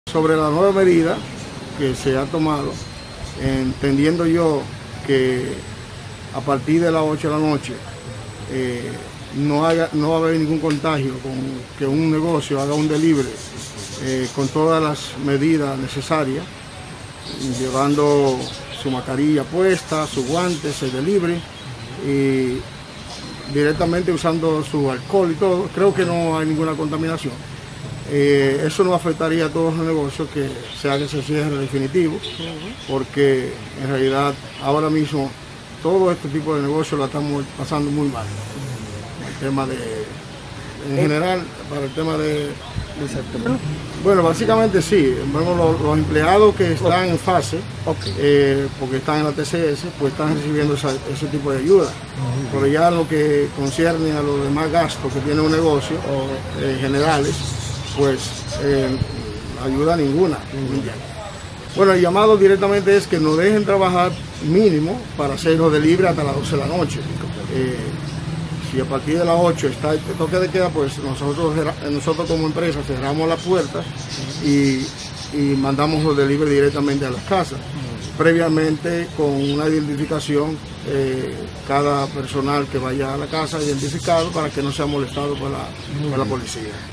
Dueño de restaurante en Tenares piden que los dejen trabajar tras la aprobación del nuevo toque
San Francisco De Macorís.- Un dueño de uno de los restaurantes del municipio de Tenares, provincia Hermanas Mirabal, llamó a las autoridades a que los dejen trabajar con el servicio a domicilio o “delivery” hasta la medianoche.

Hander Javer señala que con los servicios a domicilio y aplicando todas las medidas de seguridad, el riesgo de contagio de coronavirus sería muy mínimo, por lo que considera que pueden trabajar durante el toque de queda con una autorización a la Policía Nacional.

Indicó que la situación es muy grave para el sector, ya debido a los gastos que han tenido que incurrir “la están pasando muy mal”.